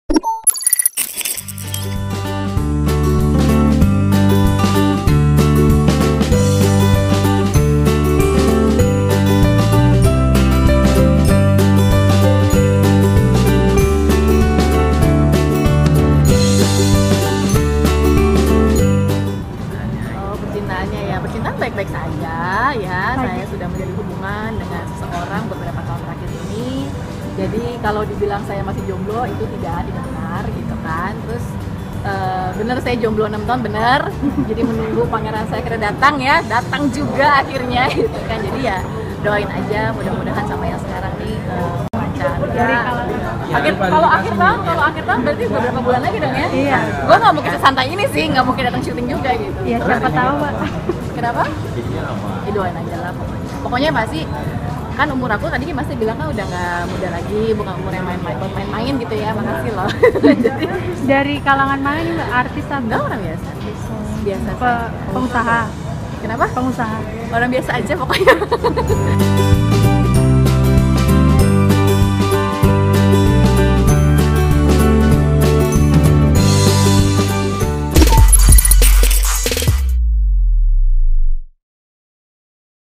Yuanita Bicara Blak-blakan Soal Pasangan Barunya
Lama tidak berpacaran, Yuanita cerita tentang pasangan barunya. Sekarang Yuanita sudah tidak single lagi nih. Pasangannya kali ini bukanlah dari kalangan artis, tapi dari orang biasa-biasa saja.